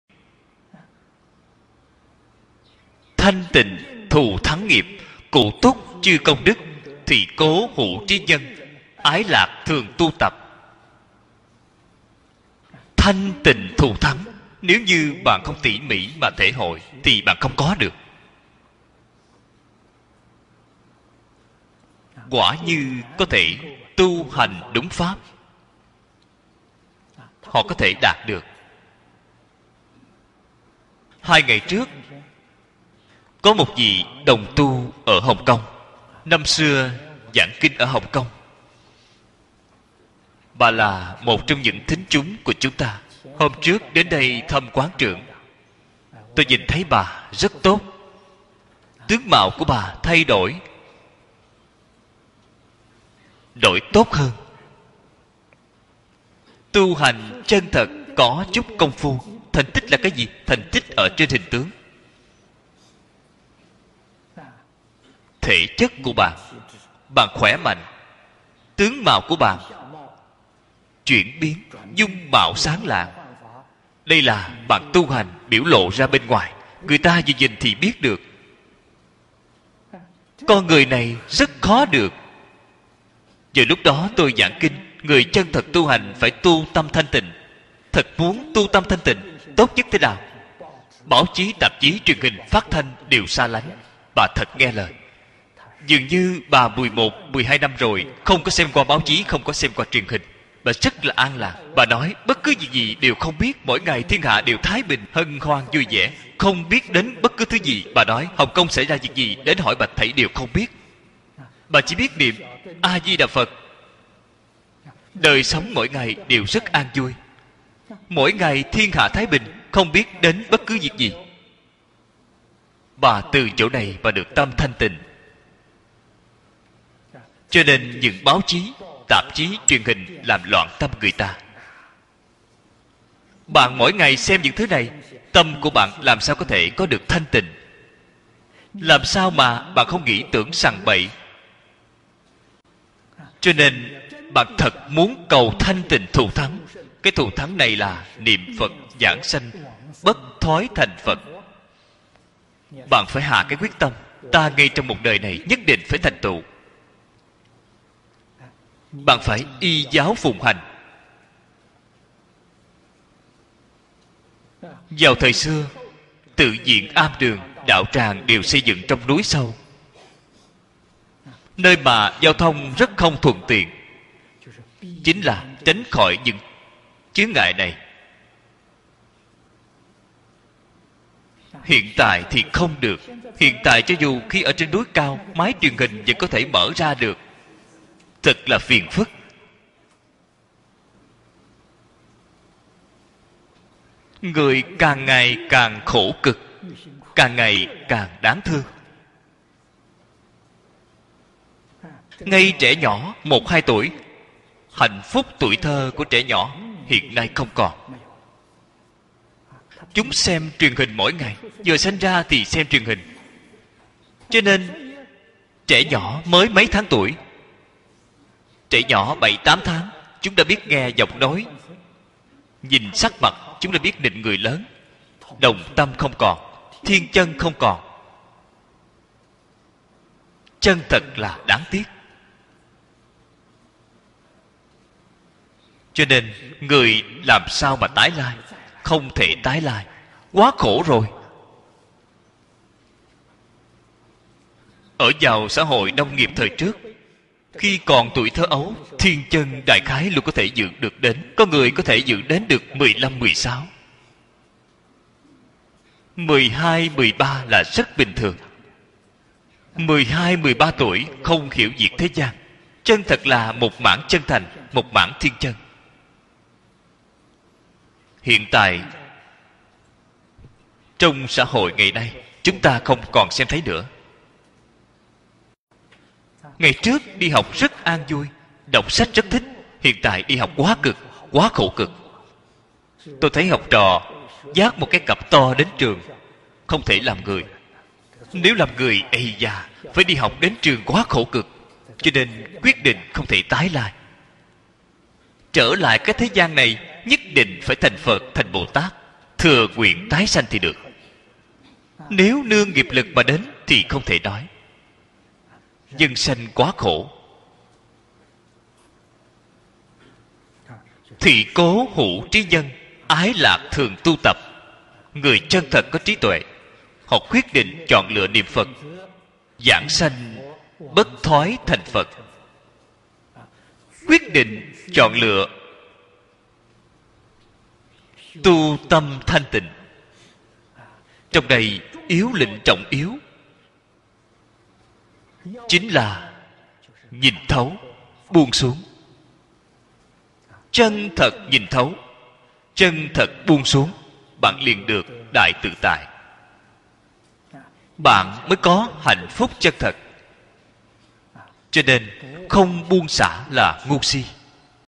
Hạnh phúc tuổi thơ của trẻ nhỏ hiện nay không còn, vì xem truyền hình mỗi ngày | T10
Trích từ buổi giảng Phát Khởi Bồ Tát Thù Thắng Chí Nhạo Kinh, tập 10
Pháp sư Tịnh Không giảng

Chánh kinh:

【清淨殊勝業。
具足諸功德。
是故有智人。
愛樂常修習。】

【Thanh tịnh thù thắng nghiệp,
Cụ túc chư công đức,
Thị cố hữu trí nhân,
Ái lạc thường tu tập.】

(Nghiệp thanh tịnh thù thắng,
Đầy đủ các công đức.
Vì thế, người có trí,
Yêu thích thường tu tập.)